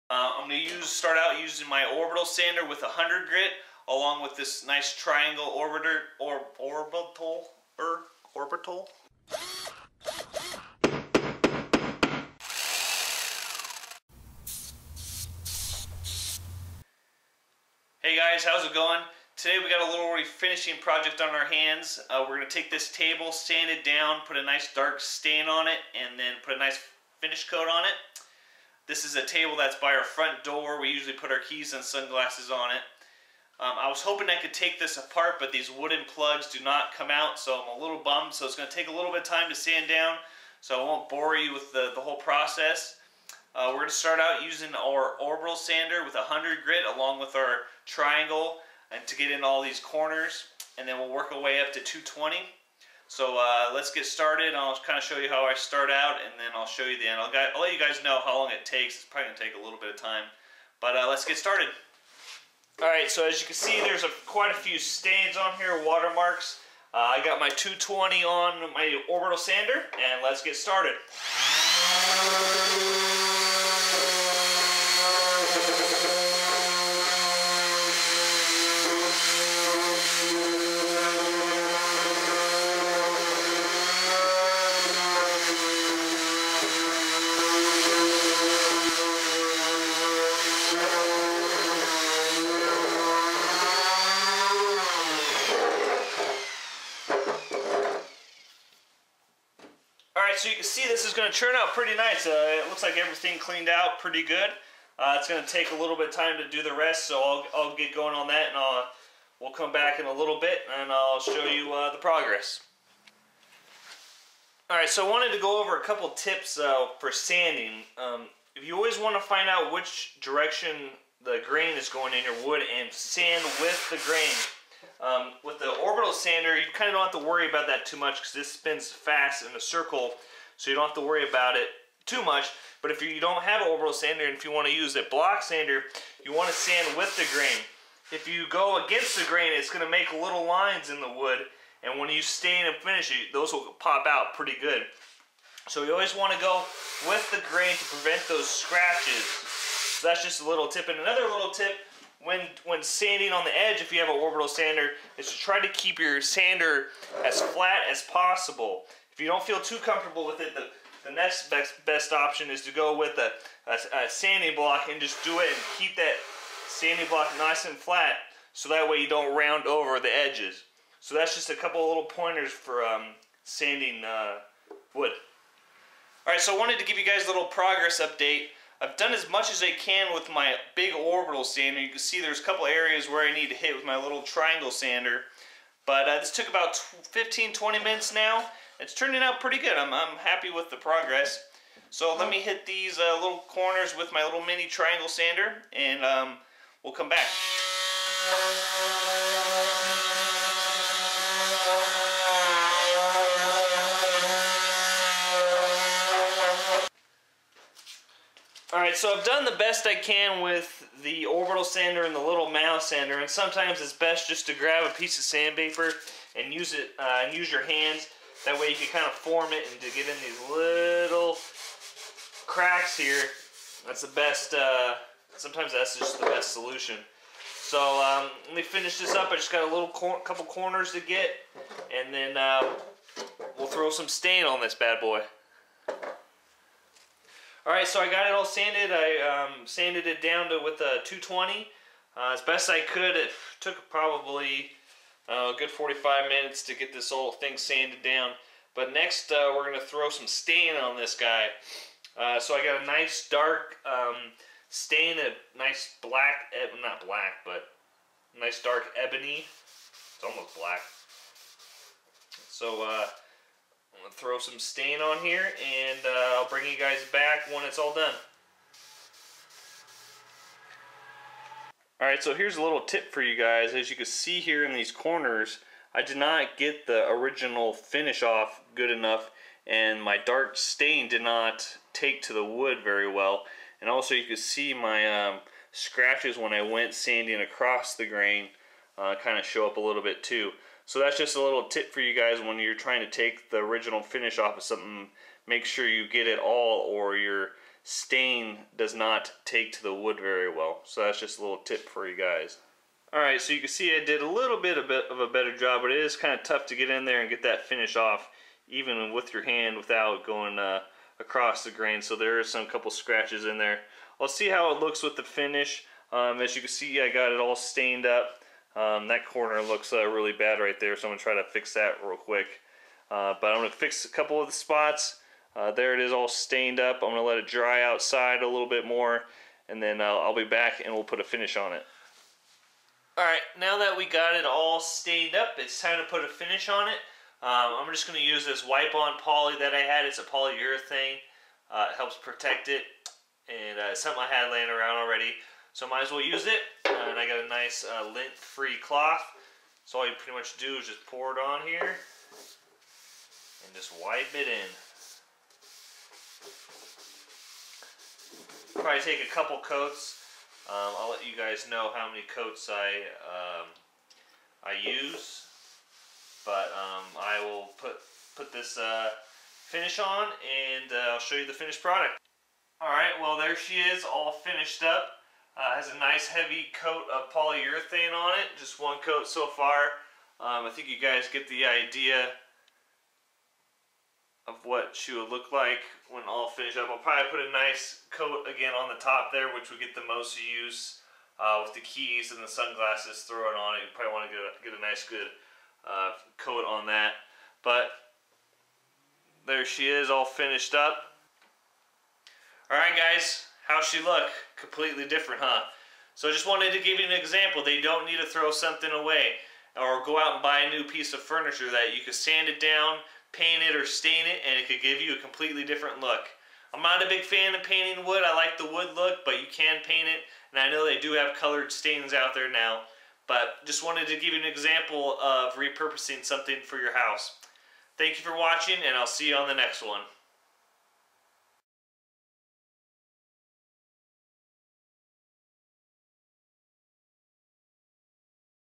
How to refinishing a small wood table
Don't throw that old pieces of furniture away. I took a small wooden table and gave it a whole new look. With a little bit of time and some elbow grease you can repurpose just about anything.